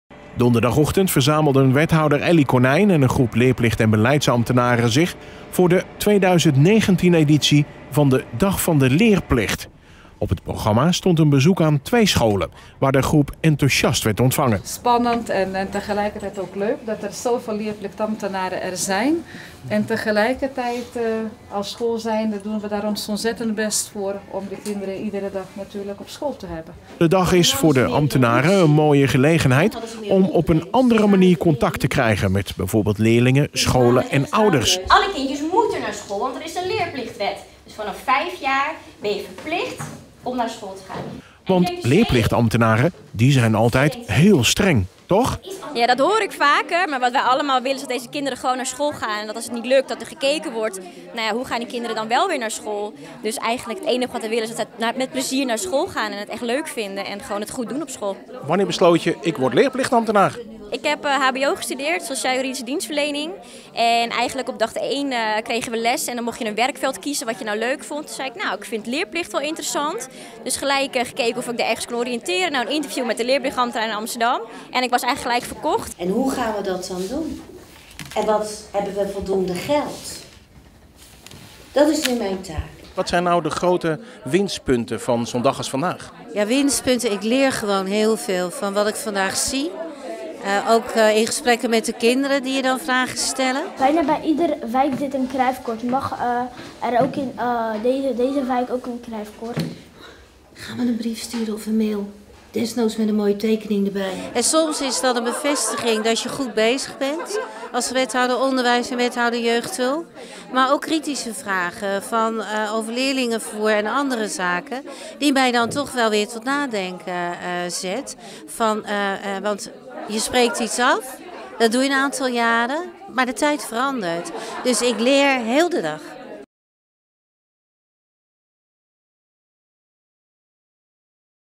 Leerzame 2019 editie van de Dag van de Leerplicht
Donderdag vond de 2019 editie van de Dag van de Leerplicht plaats. Wethouder onderwijs Elly Konijn ging daarom samen mer leerplicht- en beleidsambtenaren op bezoek bij twee Alkmaarse scholen. beide scholen bieden een aangepaste vorm van onderwijs aan en kennen daardoor weer andere uitdagingen dan 'gewone' scholen.

Wat ze daarmee juist delen is de passie voor onderwijs en de sterke wil om ieder kind zo goed voorbereid mogelijk de volgende stap te laten maken.
Voor de ambtenaren is het een mooie kans om onder andere omstandigheden in contact te komen met de kinderen, ouders en met de scholen. Normaal gesproken komt bijvoorbeeld de leerplichtambtenaar alleen in beeld wanneer er iets mis gaat.

Donderdag bleek dat hun motivatie opvallende en grote gelijkenissen vertoond met die van de juffen en meesters. Er werd gedurende de dag dan ook geanimeerd gesproken. Ook wethouder Konijn was enthousiast: Je krijgt allerlei vragen van bijvoorbeeld kinderen en ouders en dat vertelt je of je goed bezig bent, ik leer op zo'n dag nog altijd bij."